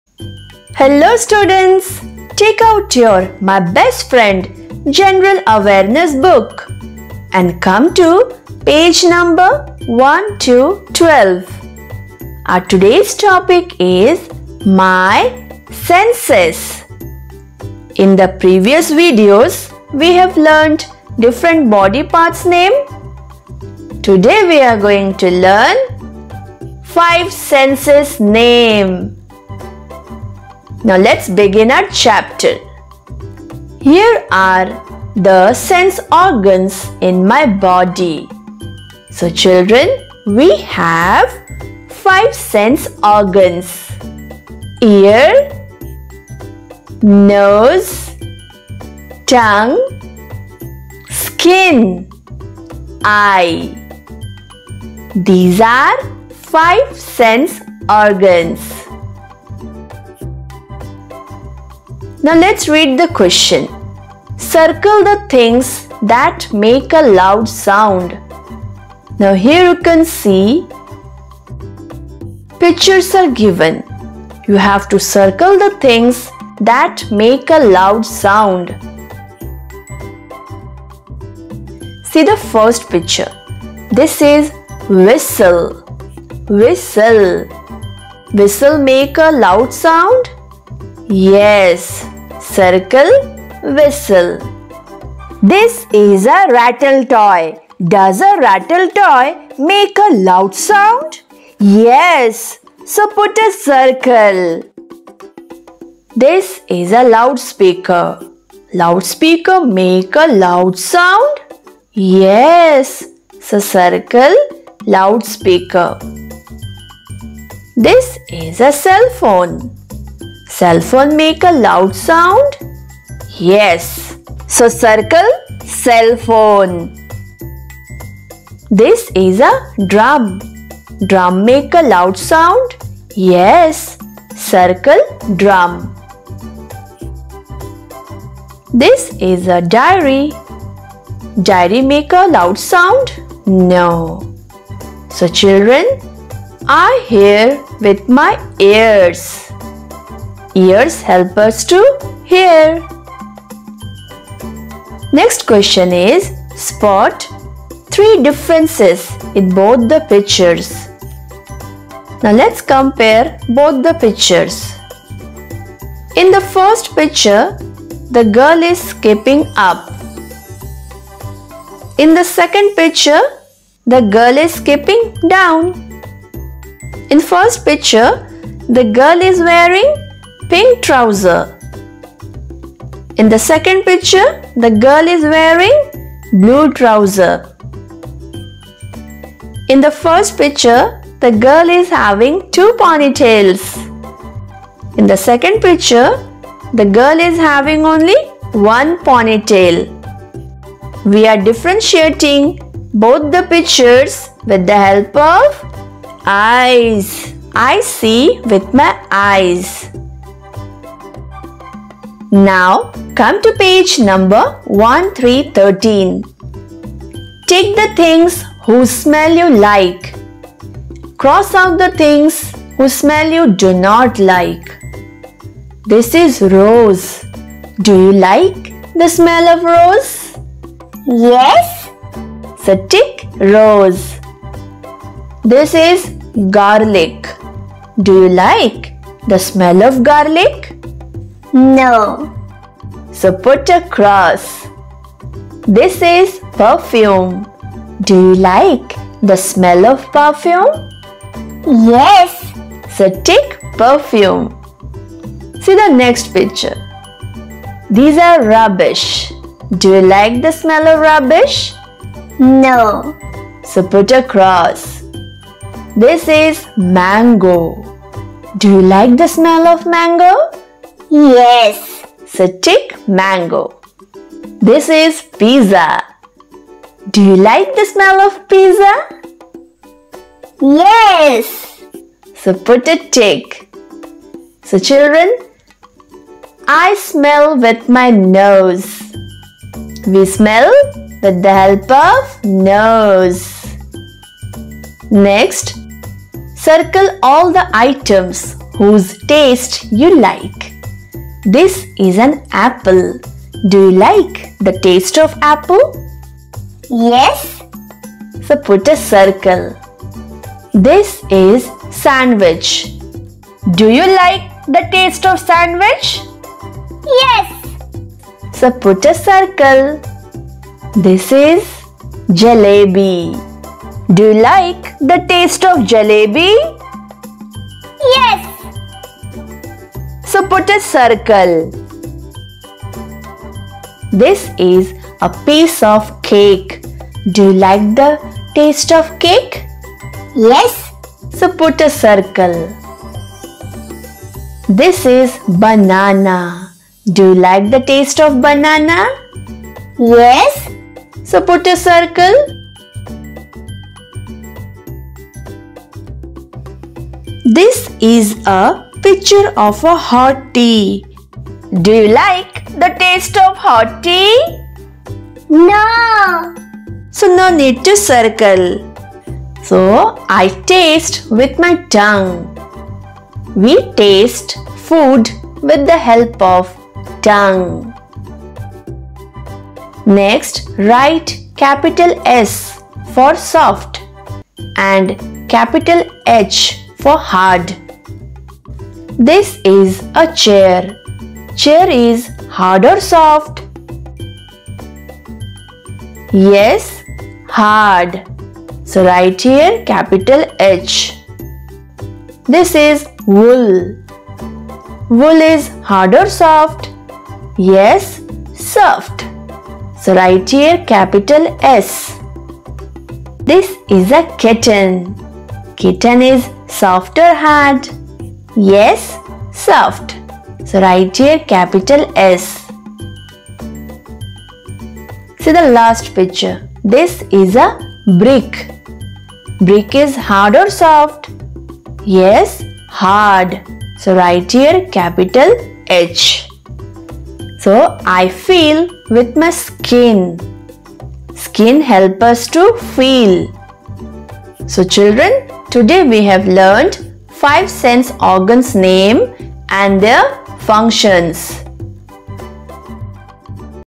Class : UKG/ My Senses
In this video you will learn about five sense organs name and their functions.